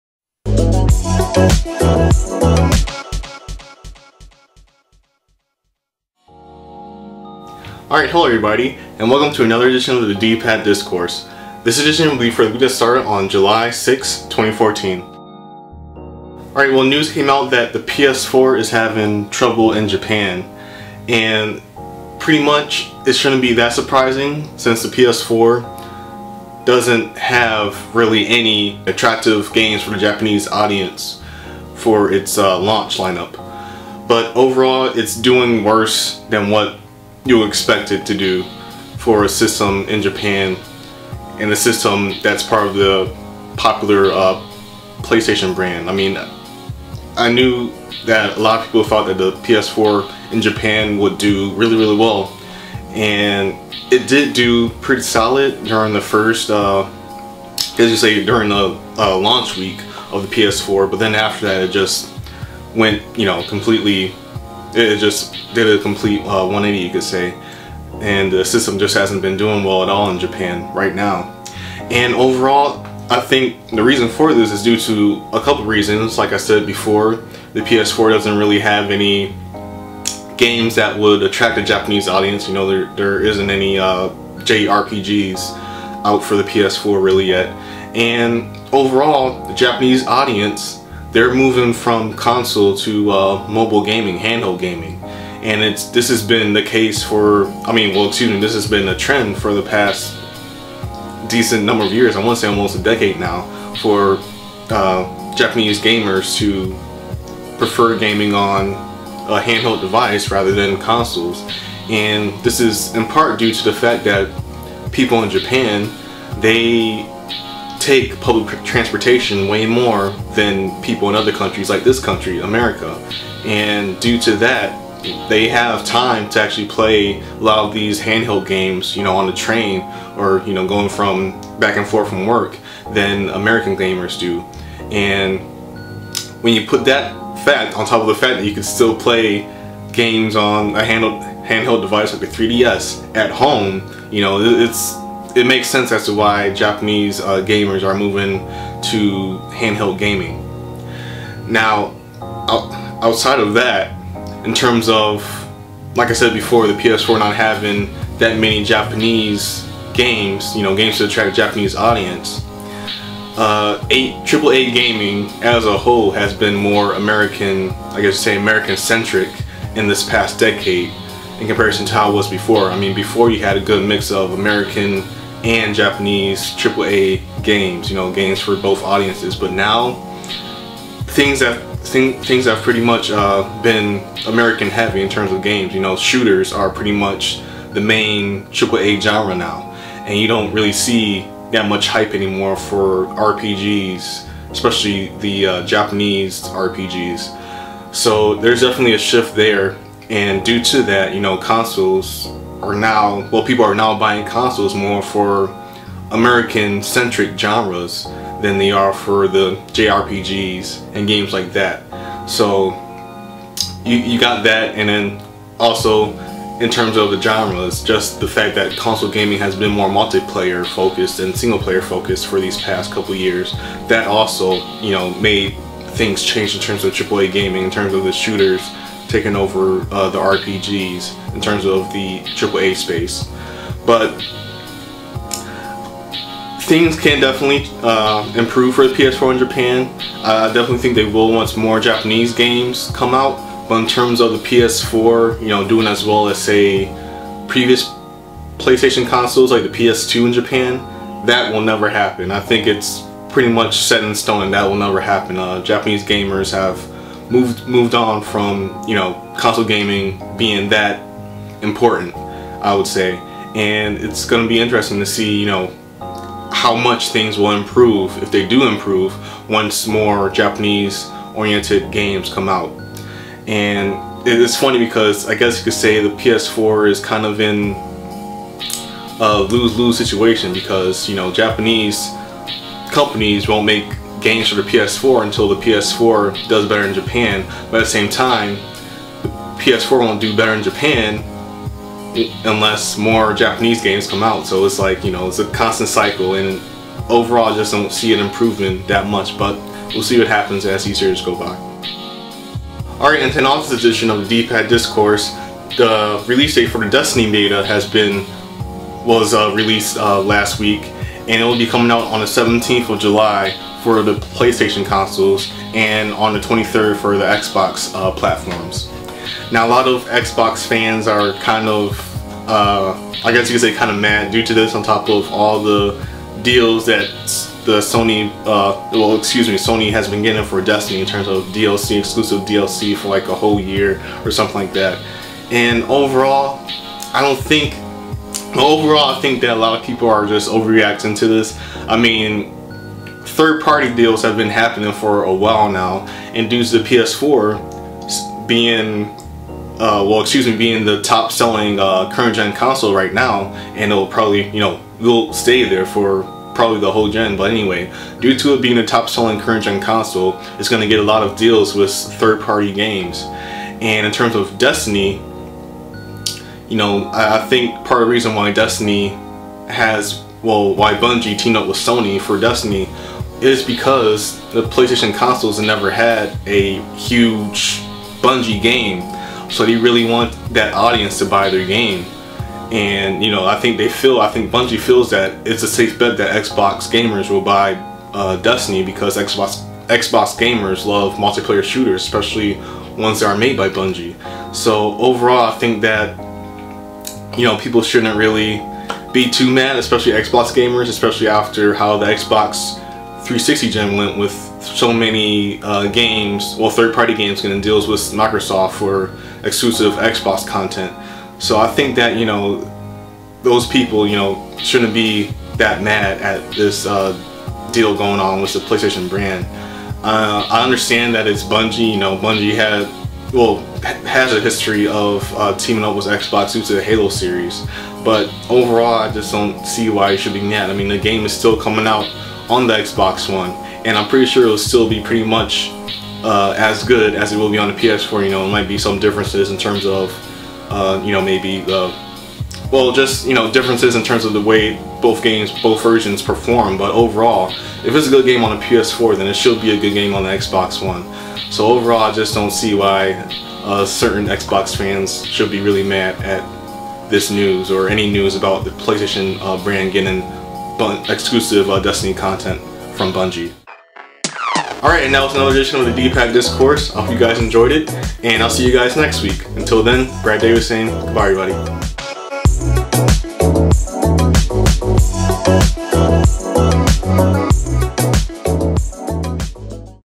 PS4 in Japan, Destiny Beta Dates & "Bungie Betrayal" ("The D-Pad Discourse")
"The D-Pad Discourse" for the week starting on July 6th 2014:
-PS4's disappointing performance in Japan
-Destiny Beta release dates
-Some xbox gamers are feeling betrayed by Bungie